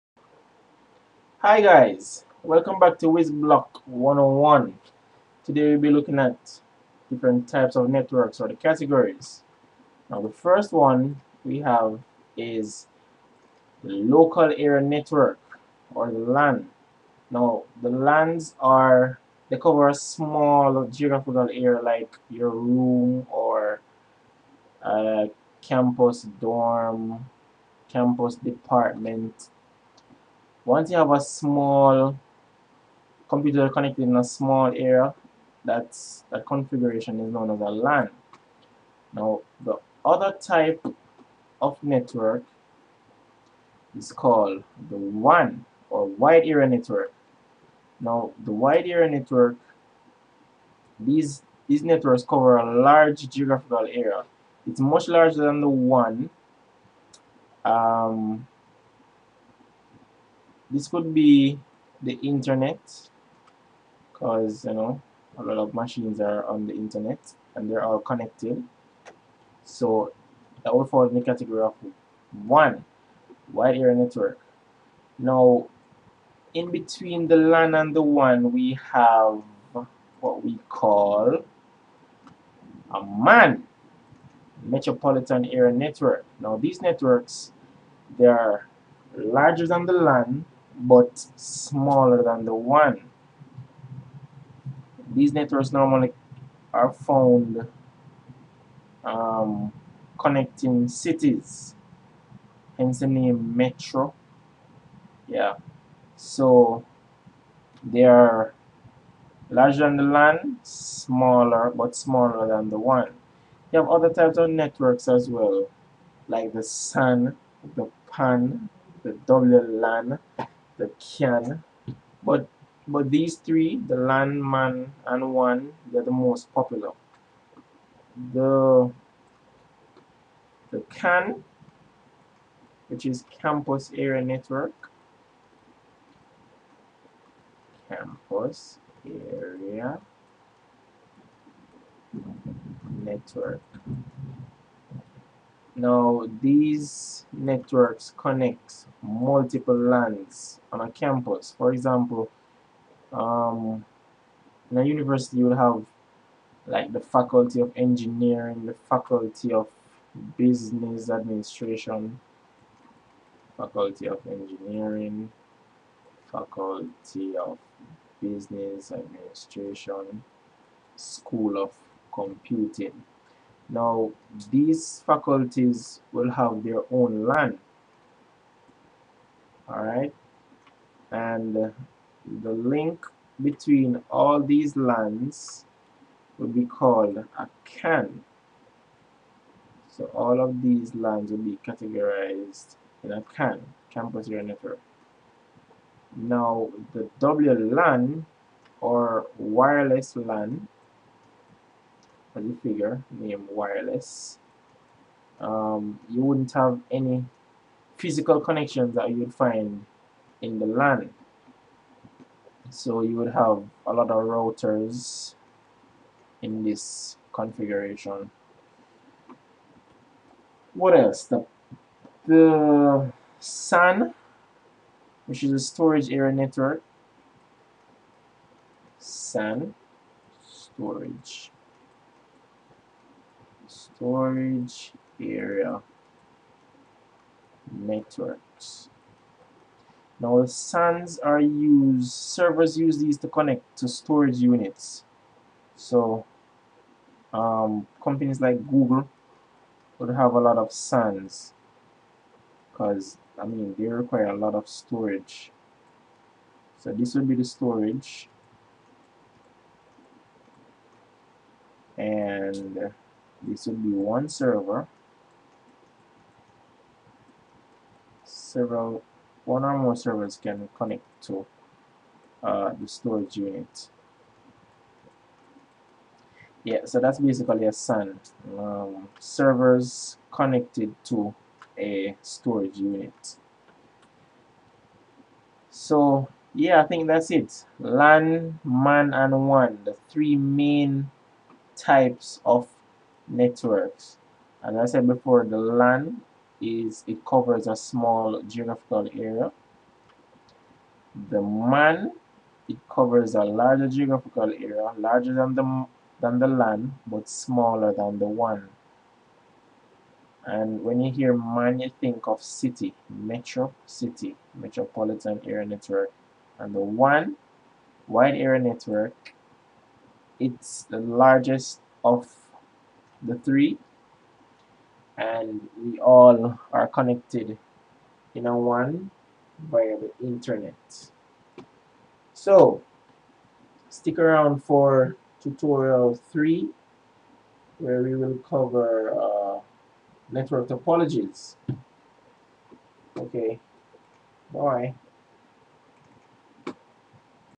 Computer Networks Turotial - 2 - Types of Networks
In this tutorial, I will introduce you to the different types of networks.